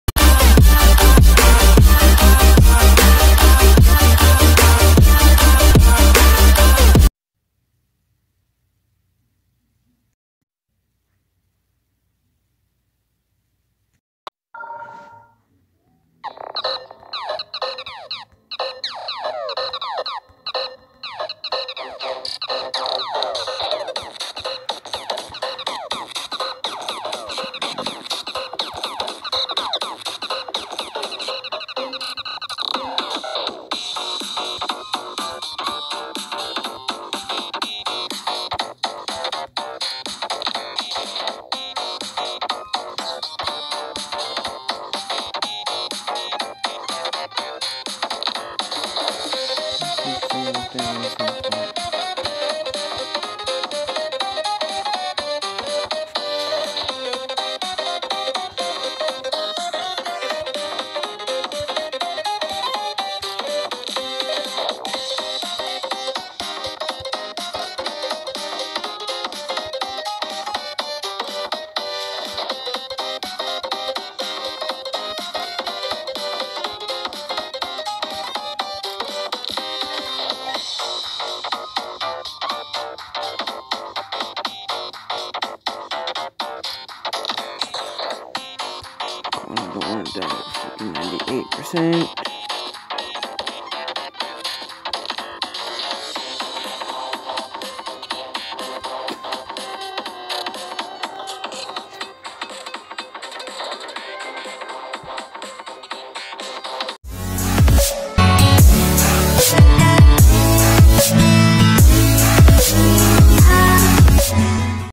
Platinum adverture by: jerry bronze v
Hello guys this is a demon that i completed a week ago so enjoy comment level requests im currently working on a demon now so yeah. Enjoy and i will catch you later.

Level name: platinum adverture
Creator:Jerry Bronze V
Rating: Easy Demon
Stars: 10
Difficulty: insane